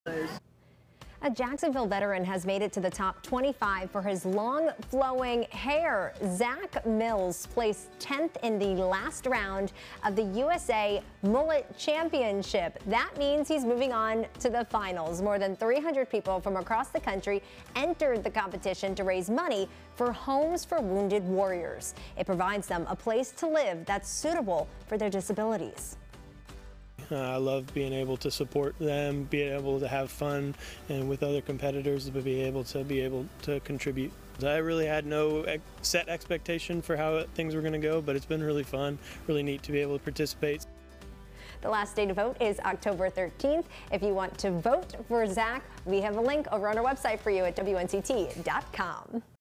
Jacksonville native makes the top 25 in USA mullet competition
One Jacksonville veteran has made it to the top 25 for his long flowing hair. You have until October 13 to vote for him in the USA Mullet Championship.